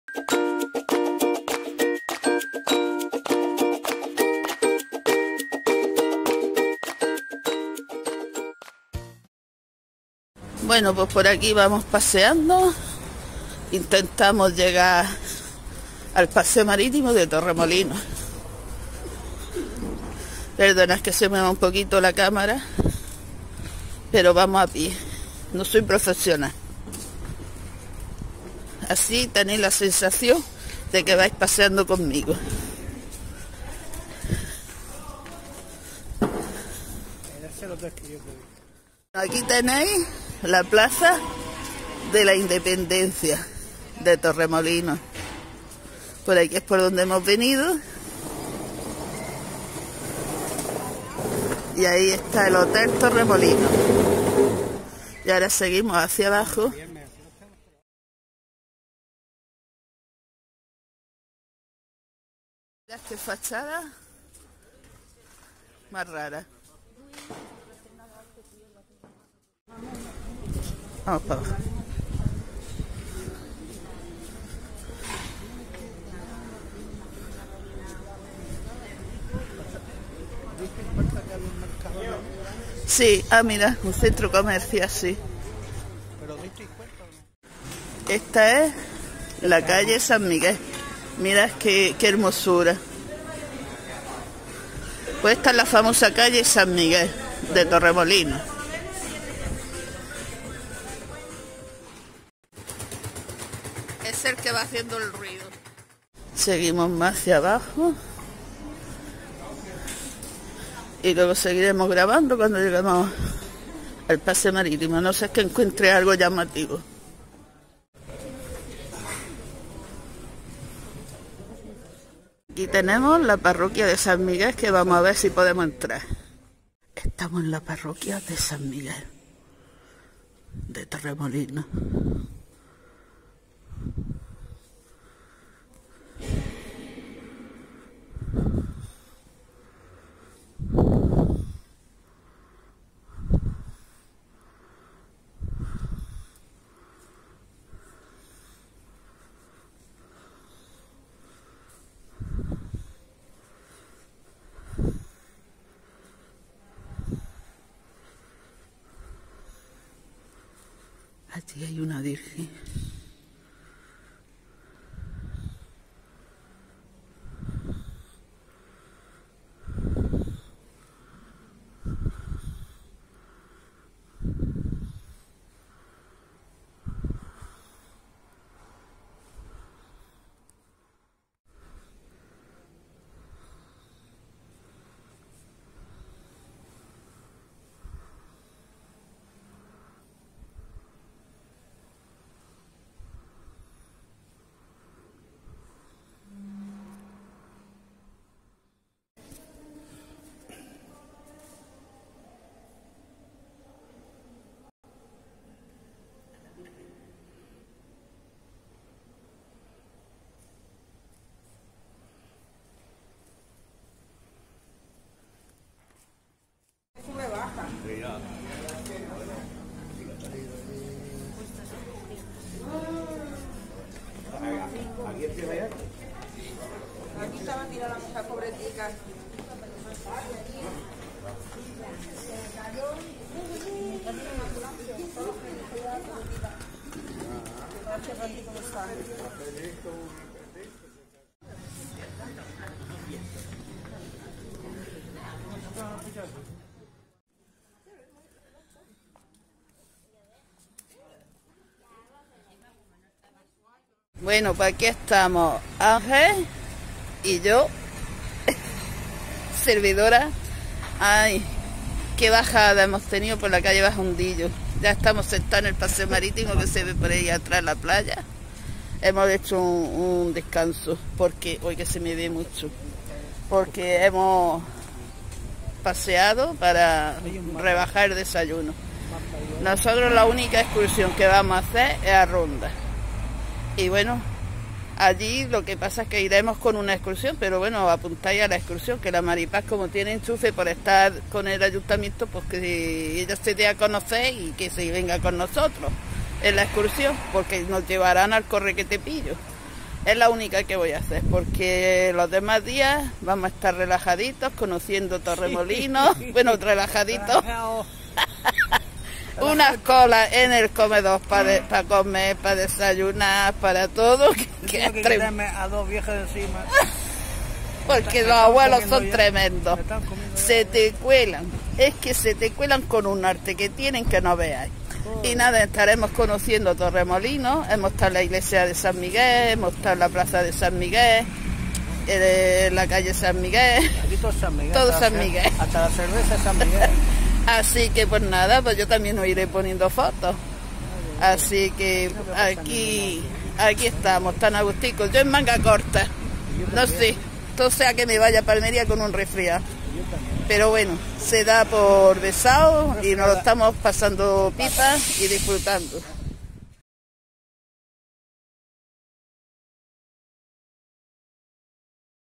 Torremolinos. Calle San Miguel. Calle Bajondillo
Ese día estuvimos paseando por la calle San Miguel,y Calle Bajondillo. Una calle muy cuqui de muchos escalones y muchas tiendas y restaurantes. Hasta que llegamos al paseo marítimo que pondré en otro video. Os espero.